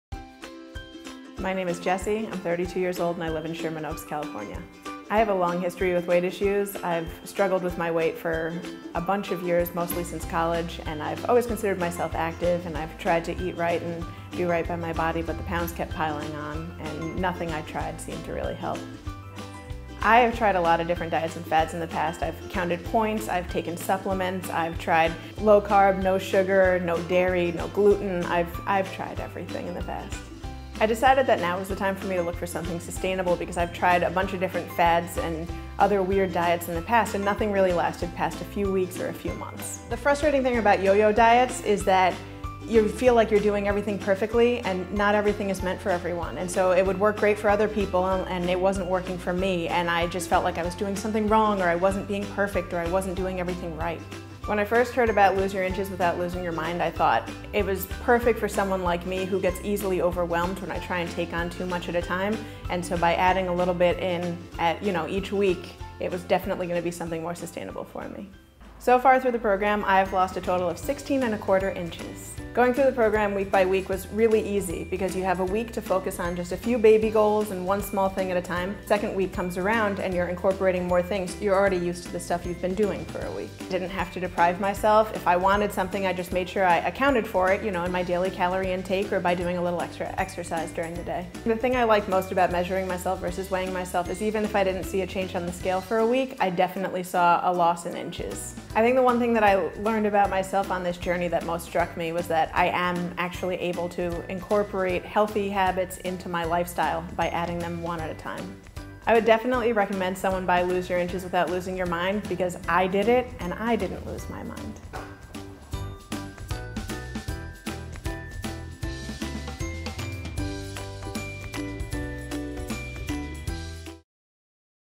Testimonial - Jessie - Lose Your Inches Program
Jessie lost over 16 inches on the Lose Your Inches Program.  Listen to her story and then order the book to start your own journey!